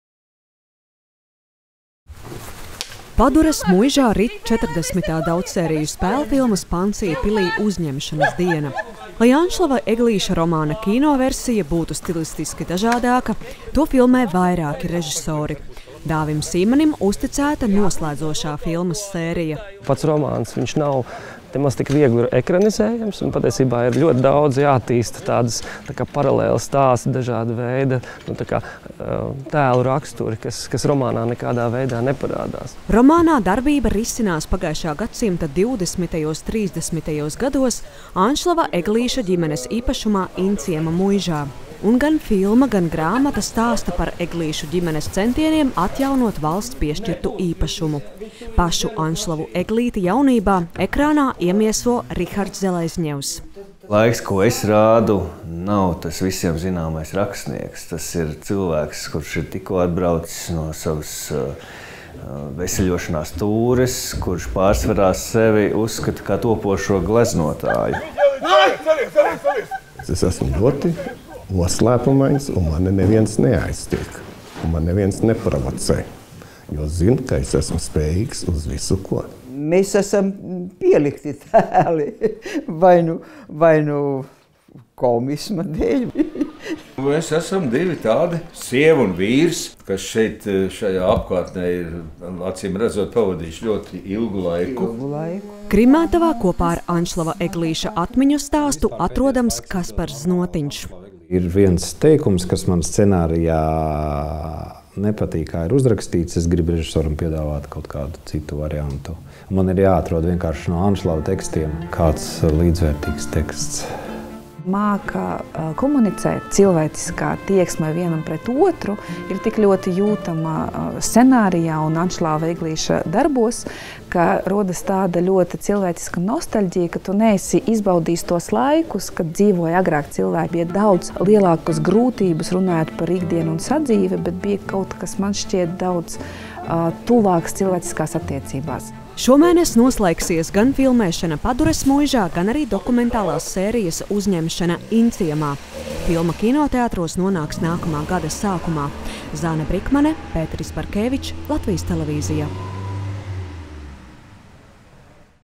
Padures muižā uzņem filmu “Pansija pilī”
Pulcējot dažādu paaudžu aktierus, skatuves un ekrāna lielmeistarus, noslēgumam tuvojas daudzsēriju spēlfilmas “Pansija pilī” uzņemšana. Kino scenārijs tapis pēc pazīstamā Anšlava Eglīša tāda paša nosaukuma romāna motīviem, tomēr filmā sašaurināts gan notikumu laiks, gan telpa. Filmēšanas laukumā viesojās Zane Brikmane.